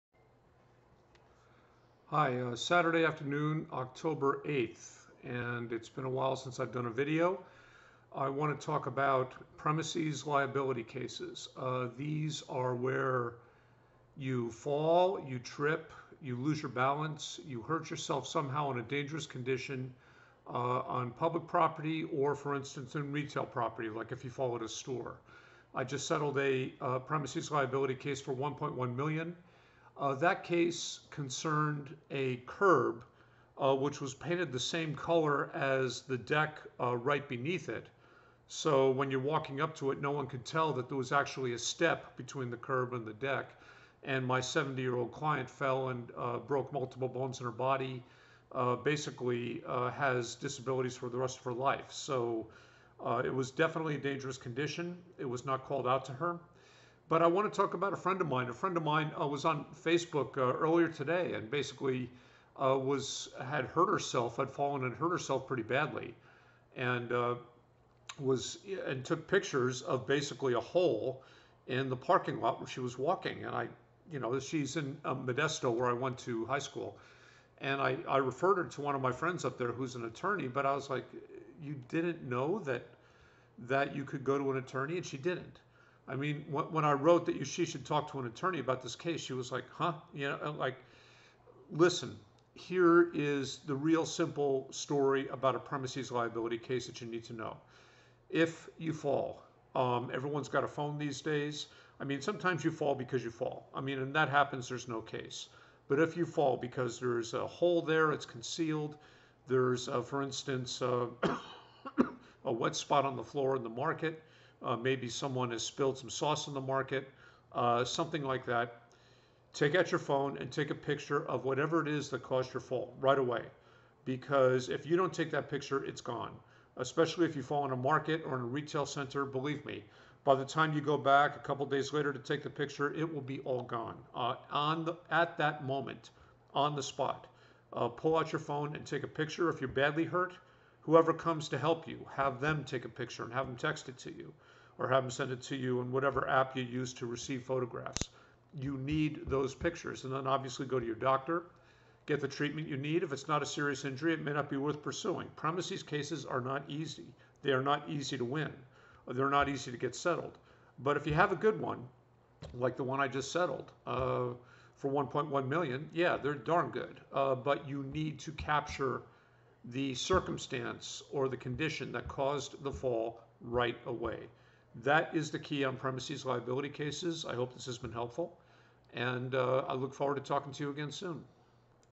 PERSONAL INJURY--PREMISES LIABILITY--TAKE A PICTURE OF THE UNSAFE CONDITION THAT CAUSED YOU TO FALL.
If you fall & hurt yourself seriously, PLEASE get your cellphone out and immediately take pictures of whatever it was that caused you to fall.  If you don't take the picture on the spot, at the time,, it won't be there a couple of days later when you return for the picture.  This could be the make or break of your case.